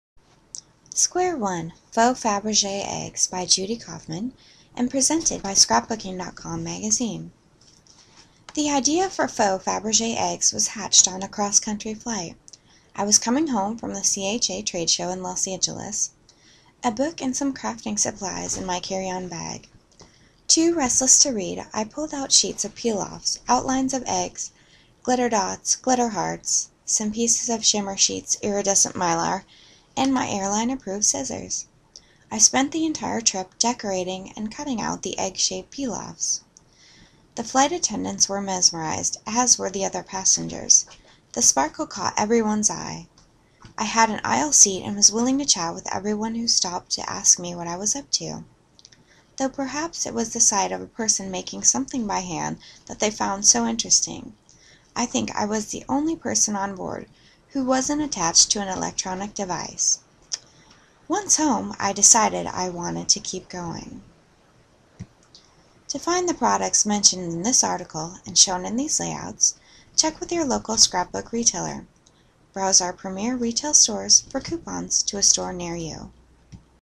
Scrapbooking.com - April 2011, Article - BasicTechniques
-- The intricate gold, enamel, and gem-encrusted designs of classic Fabergé eggs provided the inspiration for this month's Square One project.